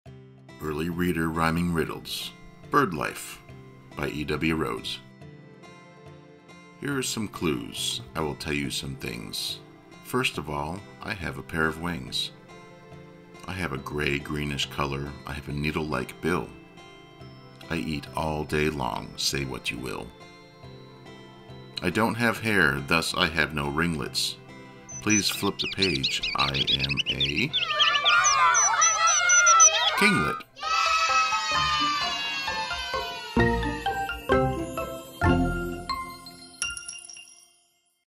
Bird Life | Part 2 | Papa Stories
Early Reader Rhyming Riddles: Bird Life by E.W. Rhodes

The Early Reader Rhyming Riddles series captivates young readers with colorful illustrations, fun challenges, interesting facts, and memorable rhymes. What better way to reinforce the reading skills of the little ones in your life than with simple rhymes that include repetitive phrases and colorful descriptions.

Read aloud with original illustrations by Bryony van der Merwe
Used with Permission by publisher Timelesstales Press